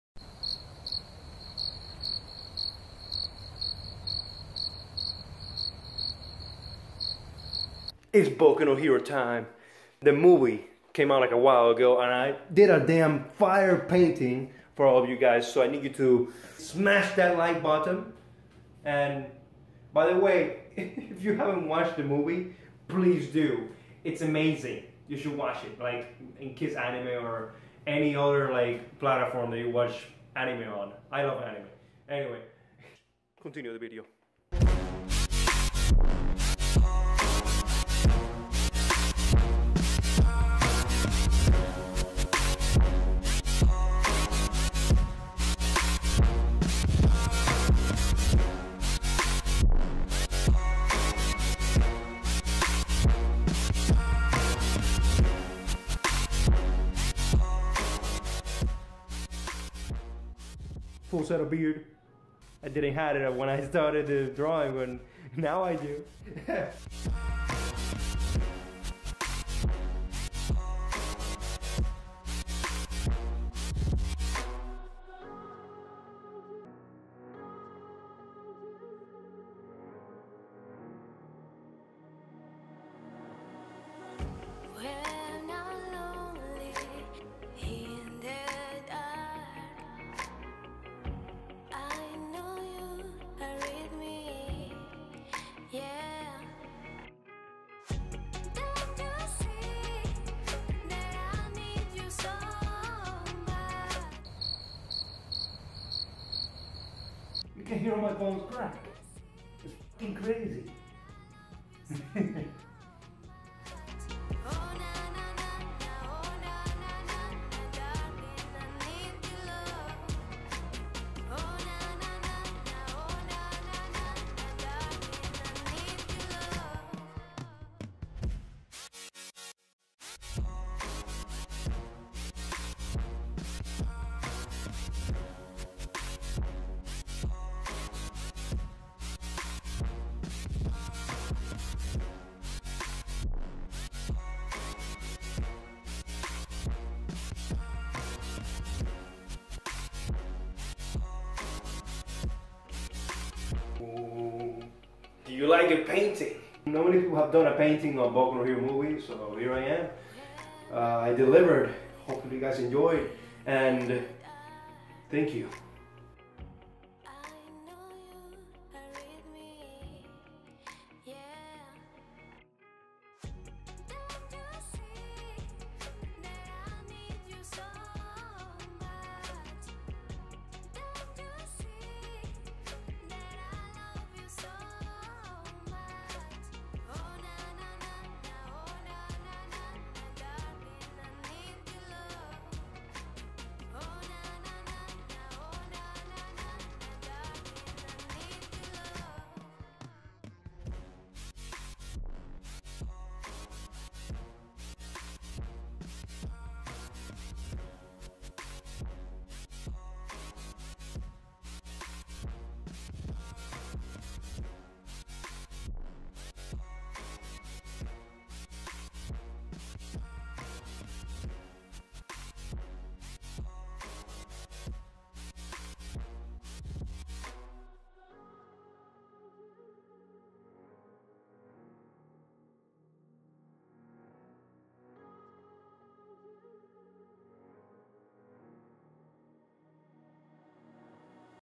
HEROES RISING *THE MOST EPIC MOMENT! (my hero academia)
Yo guys, I honestly fell in love with this movie. The moment I saw it I knew I had to make a Epic Drawing! Hopefully you guys find the video as enjoyable as it was for me to make!